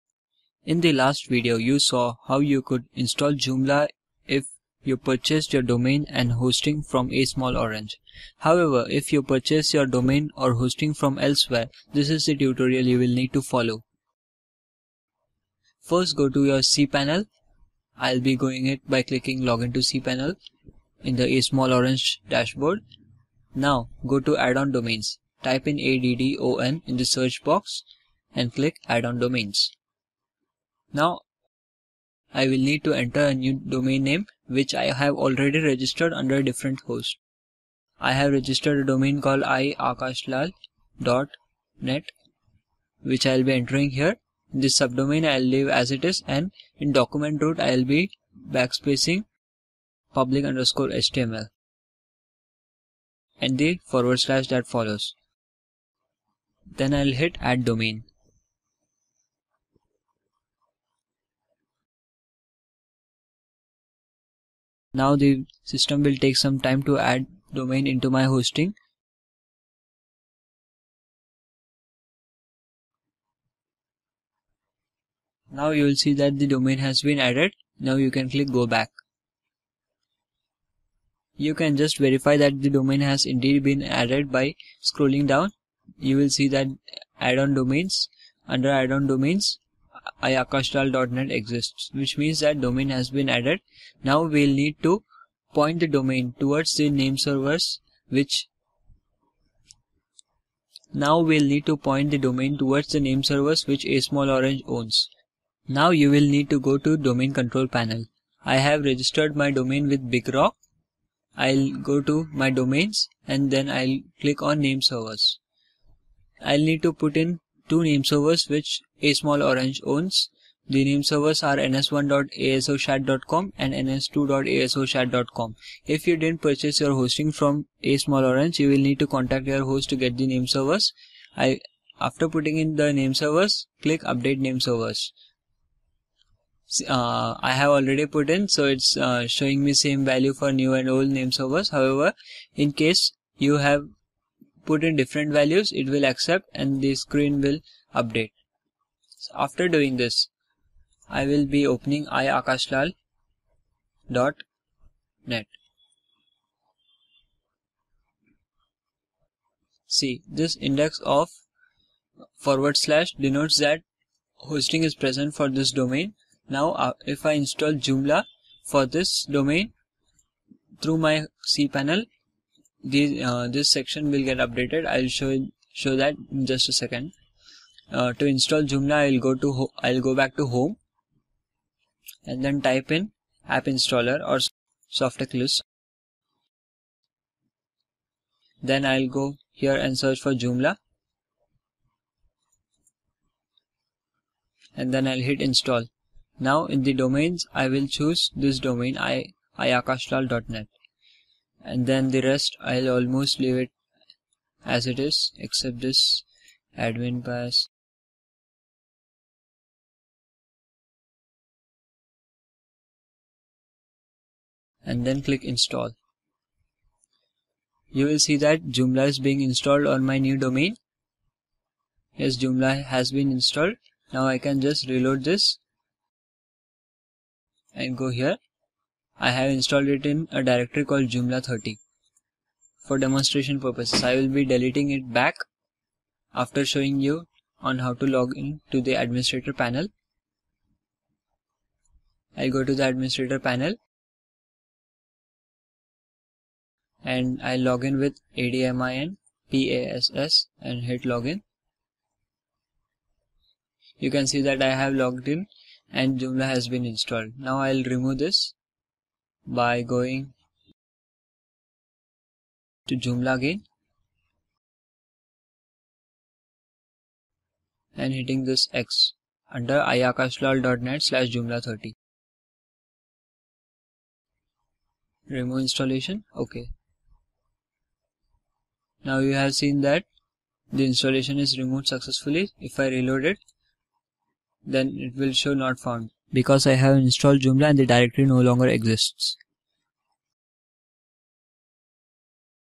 Creating Addon domain with cpanel and installing joomla - Your Joomla Tutor
This lecture is a part of tutorial series on how you can make lead collecting landing page joomla website and automate the marketing. You can access the course at our official

If you purchased domain and hosting from different registrar and host, you will need to create an addon domain in your hosting, point nameservers before installing joomla, which is what you will be seeing in this video